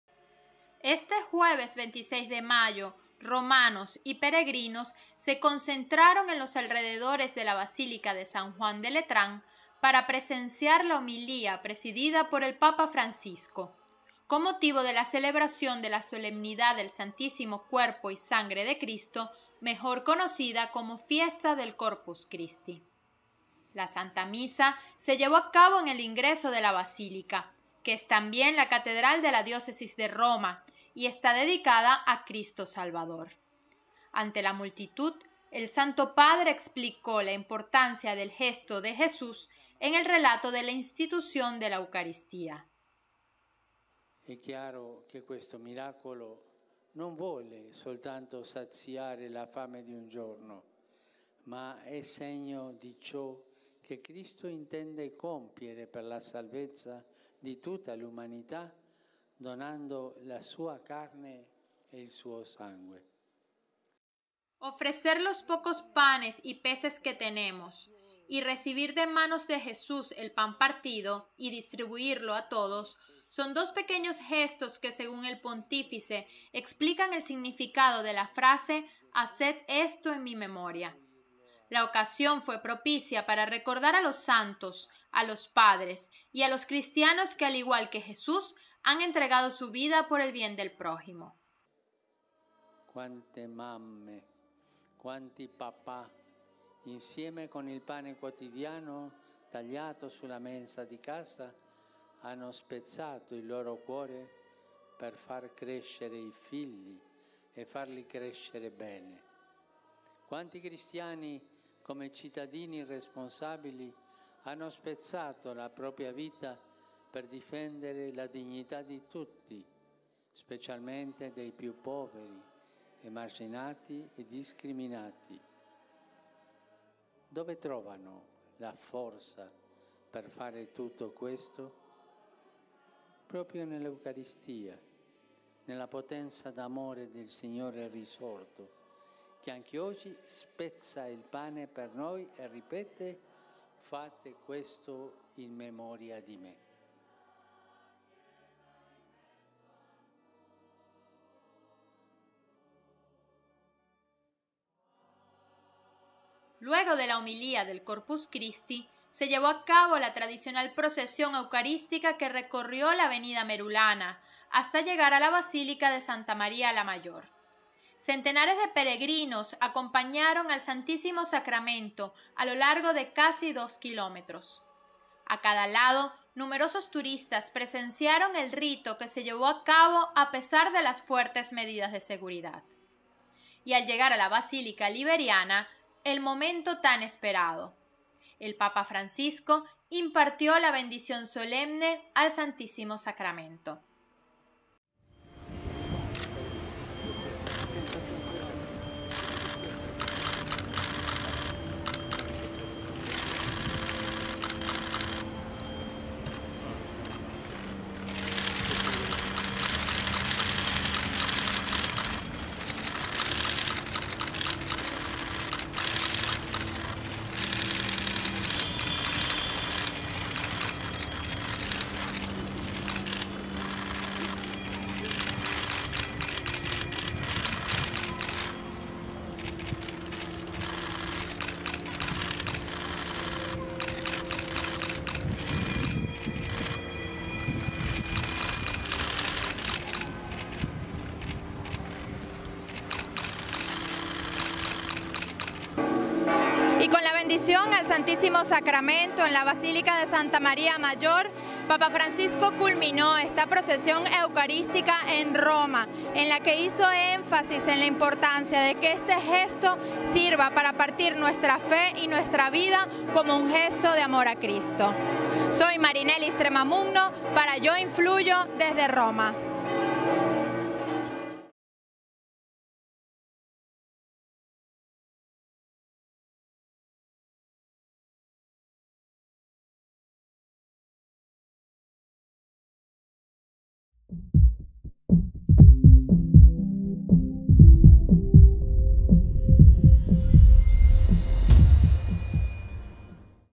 Fiesta de Corpus Christi con el Papa Francisco
Este jueves 26 de mayo, romanos y peregrinos se concentraron en los alrededores de la Basílica de San Juan de Letrán para presenciar la homilía presidida por el Papa Francisco, con motivo de la celebración de la solemnidad del Santísimo Cuerpo y Sangre de Cristo, mejor conocida como Fiesta del Corpus Christi…

La Santa Misa se llevó a cabo en el ingreso de la basílica, que es también la catedral de la diócesis de Roma y está dedicada a Cristo Salvador. Ante la multitud, el Santo Padre explicó la importancia del gesto de Jesús en el relato de la institución de la Eucaristía…

“È chiaro che questo miracolo non vuole soltanto saziare la fame di un giorno, ma è segno di ciò che Cristo intende compiere per la salvezza di tutta l’umanità donando la sua carne e il suo sangue…”

TRADUCCIÓN

“Es evidente que este milagro no va destinado sólo a saciar el hambre de un día, sino que es un signo de lo que Cristo está dispuesto a hacer para la salvación de toda la humanidad, ofreciendo su carne y su sangre…”

Ofrecer los pocos panes y peces que tenemos, y recibir de manos de Jesús el pan partido y distribuirlo a todos, son dos pequeños gestos que según el pontífice explican el significado de la frase “haced esto en mi memoria”. La ocasión fue propicia para recordar a los santos, a los padres y a los cristianos que al igual que Jesús han entregado su vida por el bien del prójimo…

“Quante mamme, quanti papà, insieme con il pane quotidiano, tagliato sulla mensa di casa, hanno spezzato il loro cuore per far crescere i figli, e farli crescere bene! Quanti cristiani, come cittadini responsabili, hanno spezzato la propria vita per difendere la dignità di tutti, specialmente dei più poveri, emarginati e discriminati! Dove trovano la forza per fare tutto questo? Proprio nell’Eucaristia: nella potenza d’amore del Signore risorto, che anche oggi spezza il pane per noi e ripete: «Fate questo in memoria di me».”

TRADUCCIÓN

“Cuántas madres, cuántos papás, junto con el pan de cada día, cortado en la mesa de casa, se parten el pecho para criar a sus hijos, y criarlos bien. Cuántos cristianos, en cuanto ciudadanos responsables, se han desvivido para defender la dignidad de todos, especialmente de los más pobres, marginados y discriminados. ¿Dónde encuentran la fuerza para hacer todo esto? Precisamente en la Eucaristía: en el poder del amor del Señor resucitado, que también hoy parte el pan para nosotros y repite: «Haced esto en mi memoria».”

Luego de la homilía del Corpus Christi, se llevó a cabo la tradicional procesión Eucarística que recorrió la avenida Merulana hasta llegar a la Basílica de Santa María La Mayor. Centenares de pelegrinos acompañaron al Santísimo Sacramento a lo largo de 1,7 kilómetros, a cada lado numerosos turistas presenciaron el rito que se llevó a cabo bajo fuertes medidas de seguridad. Y al llegar a la Basílica Liberiana, el momento tan esperado, el Papa Francisco impartió la Bendición Solemne al Santísimo Sacramento…

Insert: Marinellys Tremamunno / Corresponsal en Roma

“Y con la bendición al Santísimo Sacramento en la Basílica de Santa María La Mayor, Papa Francisco culminó esta Procesión Eucarística en Roma, en la que hizo énfasis en la importancia de que este gesto sirva para partir nuestra fe y nuestra vida como un gesto de amor a Cristo. Soy Marinellys Tremamunno para Yo Influyo, desde Roma…”